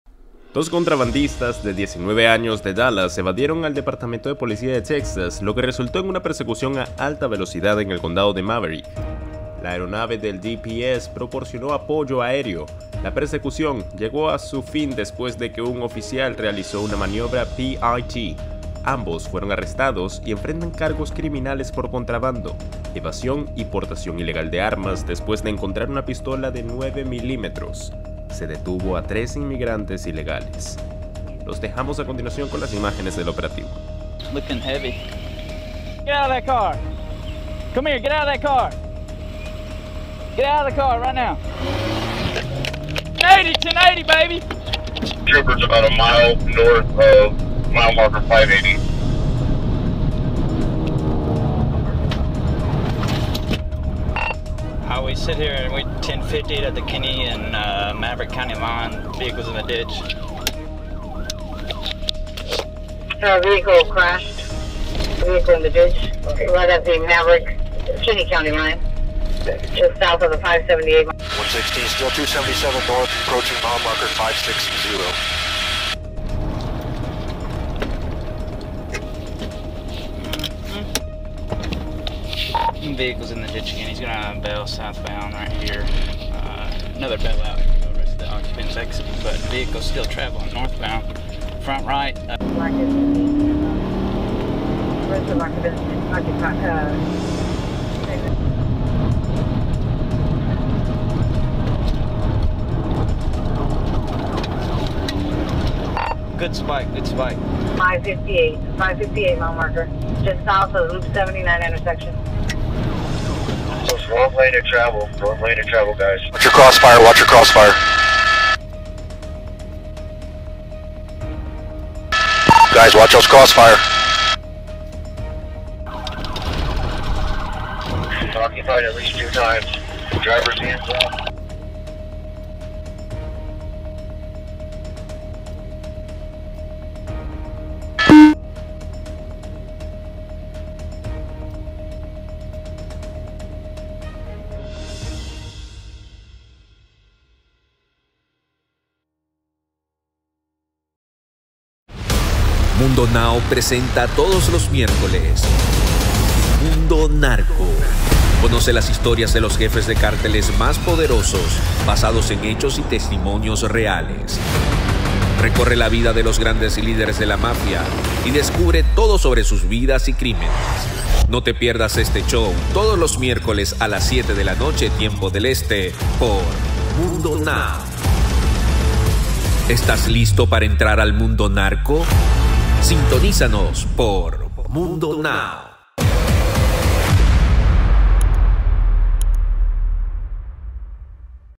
Persecución y Arresto en Texas: Operativo Policial en Vivo
Captura la intensa persecución policial en el condado de Maveric, Texas, que involucra a contrabandistas y la detención de inmigrantes ilegales. Incluye imágenes de apoyo aéreo y detención en carretera. Ve el emocionante operativo con armas y el K9 en acción. Ideal para aquellos interesados en la ley y el orden, las tácticas policiales y la seguridad pública.